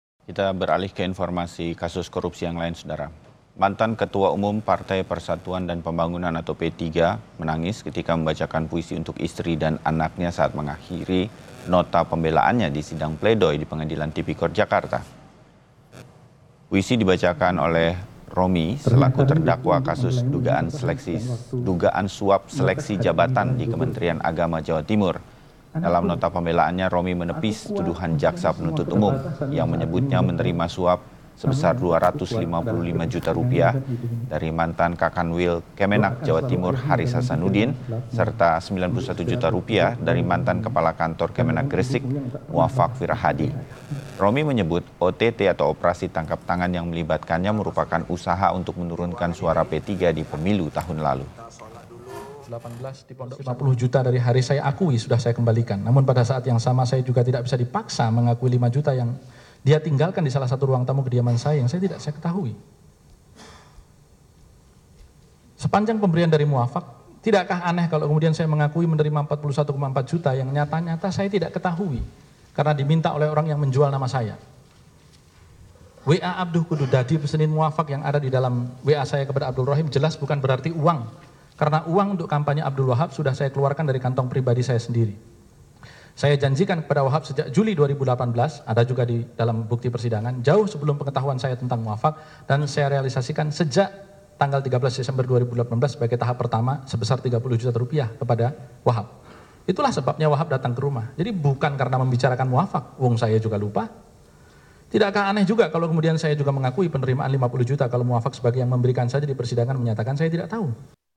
Baca Puisi untuk Istri dan Anaknya di Sidang Pleidoi, Romahurmuziy Menangis
Mantan Ketua Umum Partai Persatuan dan Pembangunan (PPP) Romahurmuziy menangis, ketika membacakan puisi untuk istri dan anaknya saat mengakhiri nota pembelaaannya di sidang pleidoi di Pengadilan Tipikor Jakarta.

Puisi dibacakan Romy selaku terdakwa kasus dugaan suap seleksi jabatan di Kementerian Agama Jawa Timur.

Dalam nota pembelaan, Romy menepis tuduhan jaksa penuntut umum yang menyebutnya menerima suap sebesar, 255 juta rupiah dari Mantan Kakanwil Kemenag Jawa Timur, Haris Hasanuddin, serta 91 juta rupiah dari Mantan Kepala Kantor Kemenag Gresik, Muafaq Wirahadi.

Romy menyebut, OTT yang melibatkannya merupakan usaha menurunkan suara PPP di pemilu tahun lalu.